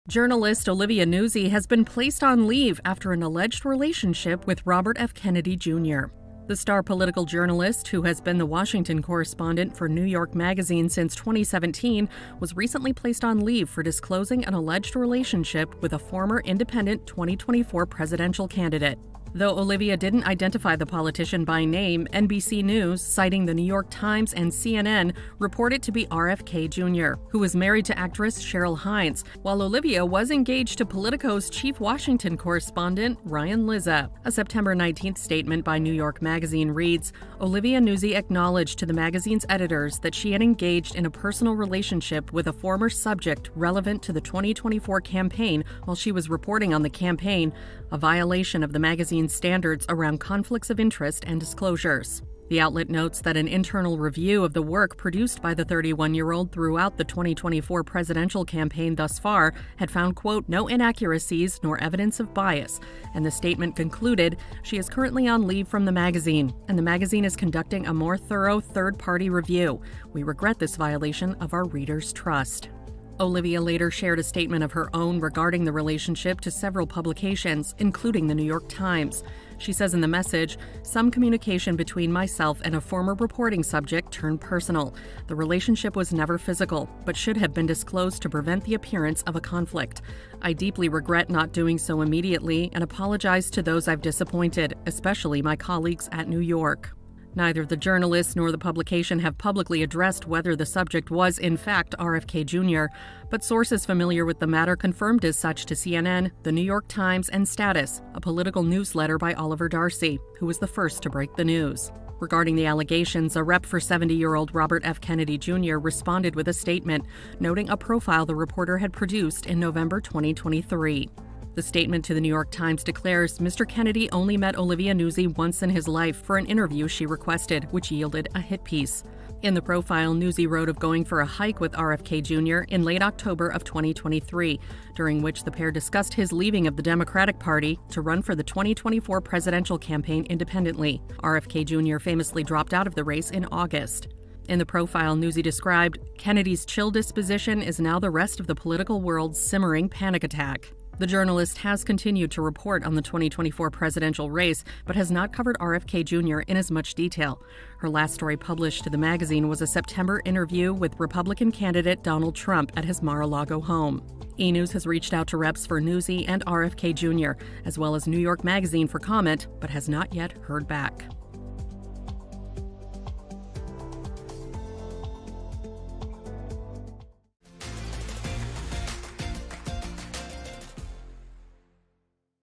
Journalist Olivia Nuzzi Placed on LEAVE After Alleged Robert F. Kennedy Jr. Relationship | E! News
New York magazine’s Washington Correspondent Olivia Nuzzi was placed on leave after disclosing an alleged “personal” relationship with Robert F. Kennedy Jr., who is married to actress Cheryl Hines. Olivia Nuzzi later shared a statement of her own, saying, "Some communication between myself and a former reporting subject turned personal. The relationship was never physical but should have been disclosed to prevent the appearance of a conflict. I deeply regret not doing so immediately and apologize to those I’ve disappointed, especially my colleagues at New York.”

Connect with E! News:
Shop E! Insider picks on Amazon